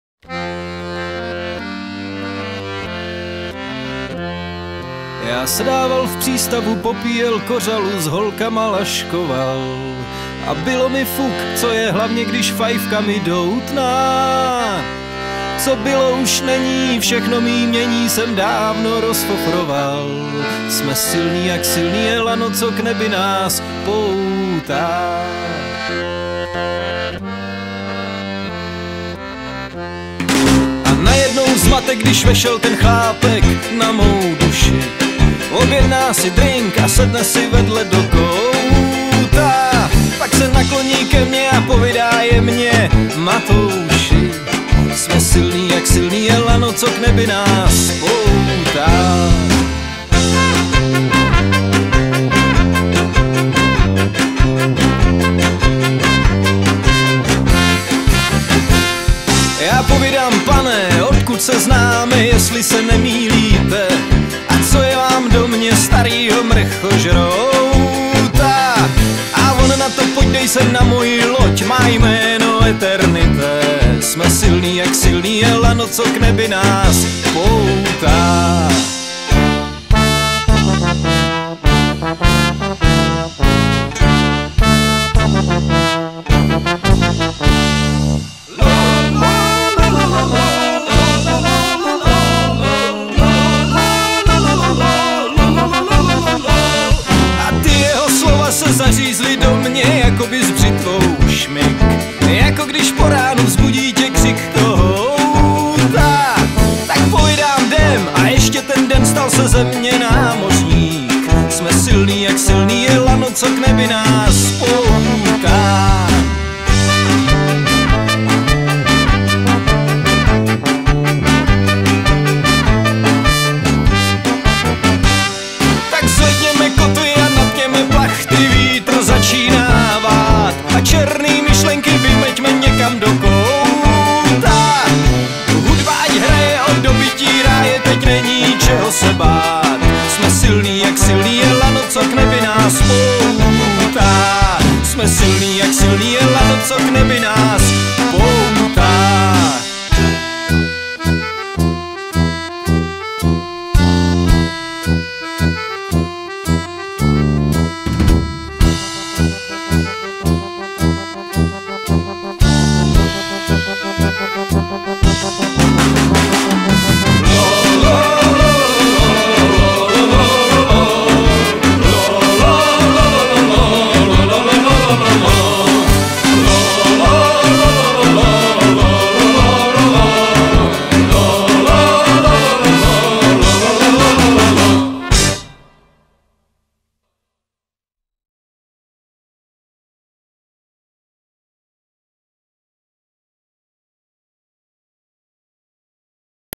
GrupaVrdy Silvestry 2004-2007
Sestřih ze společně trávených Silvestrů od roku 2004 do roku 2007 :-)  hudba Traband